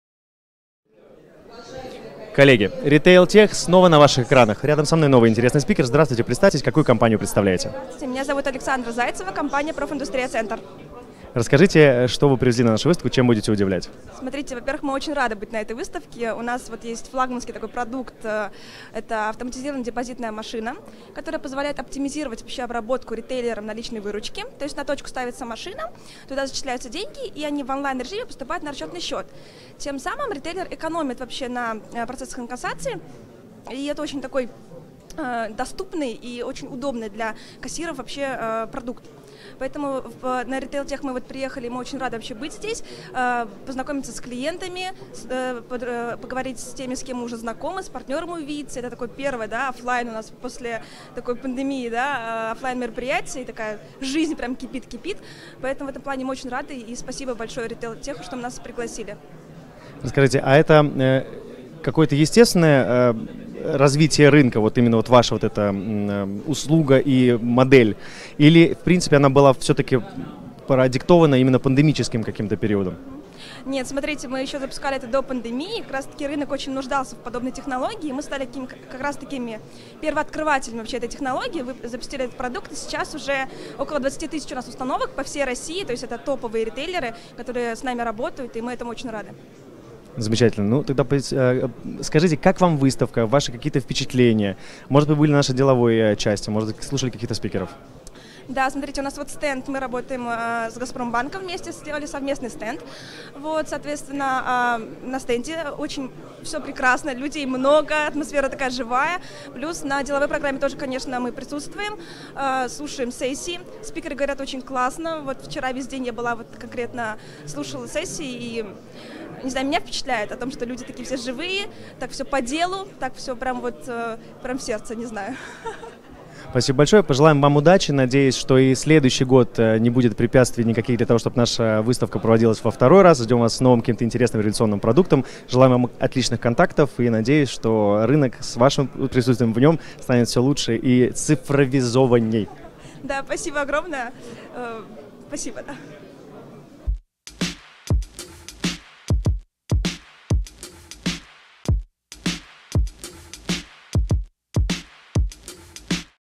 Александра Зайцева, Profindustry
Александра Зайцева, Profindustry, об автоматизированной депозитной машине и том, как оптимизировать обработку наличной выручки и экономии на инкассации